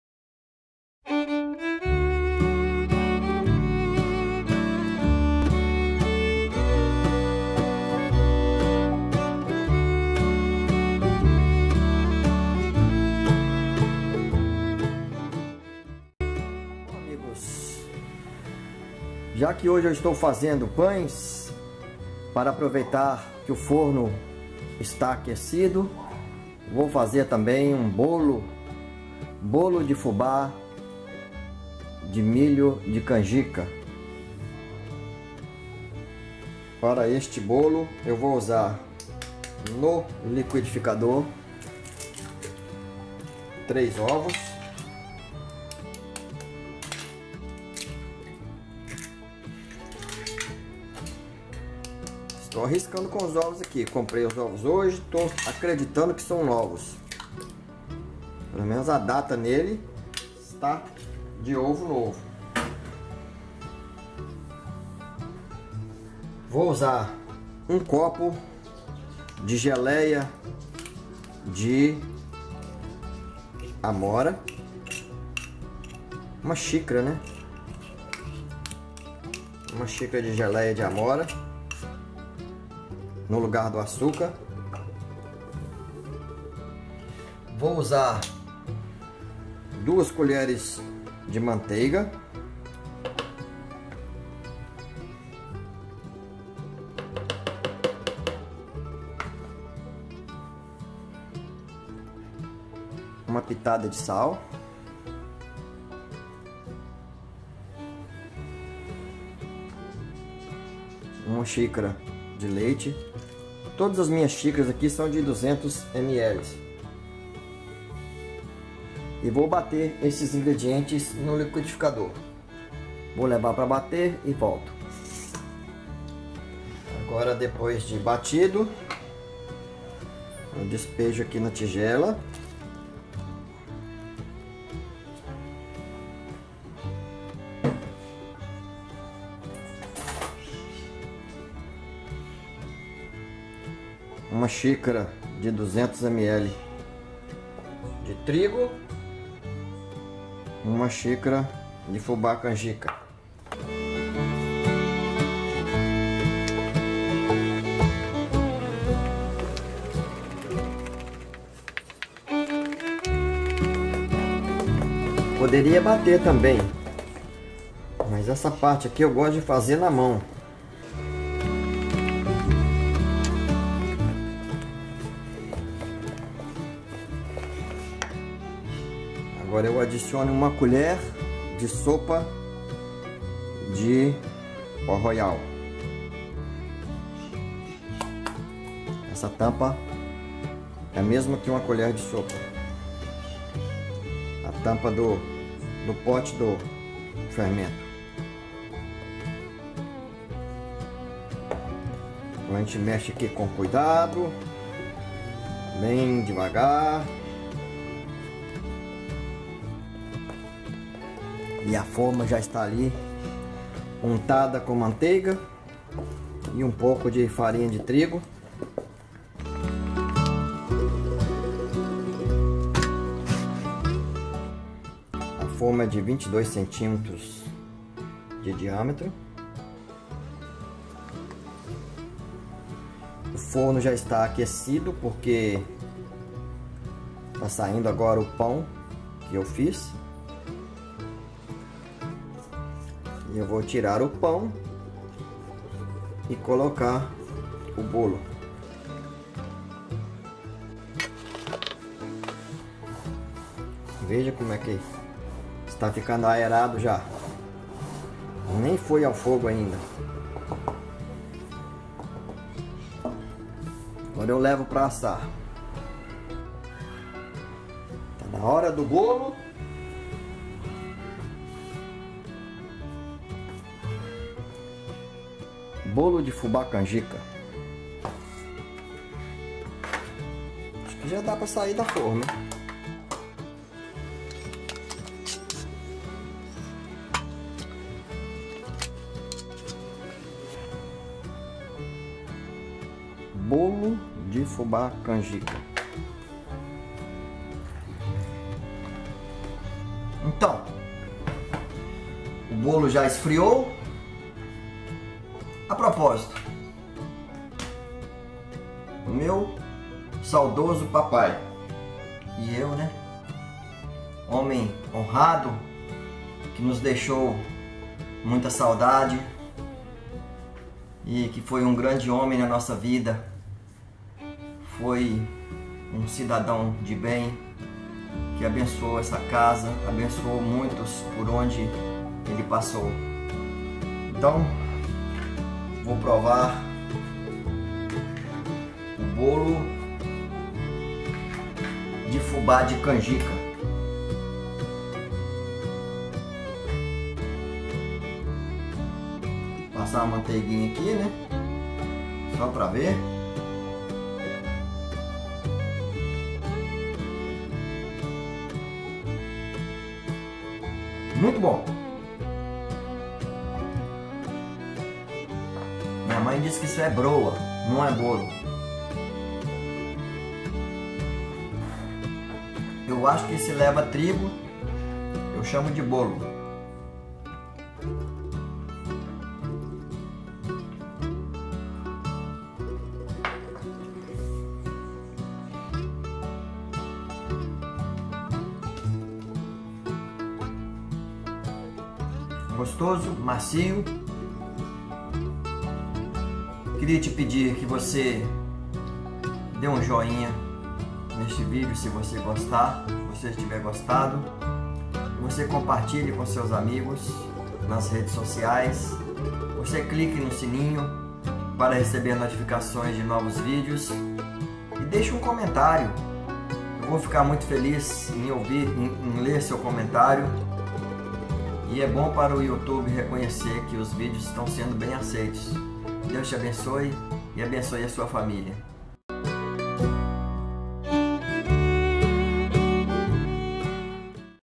Bolo de Fubá Canjica
Ingredientes:
- 3 ovos
- 1 xícara de geleia de amora
- 2 colheres de manteiga
- 1 pitada de sal
- 1 xícara de leite
- 1 xícara de farinha de trigo
- 1 xícara de fubá  de milho canjica
- 1 colher de sopa de fermento químico (pó royal)

Posso todas as coisas naquele que me fortalece.